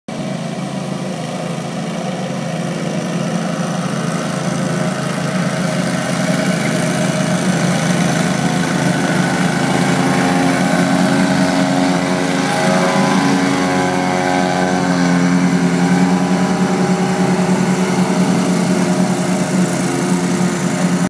Mowing in the Rain
Walker Mowers mow in rain or shine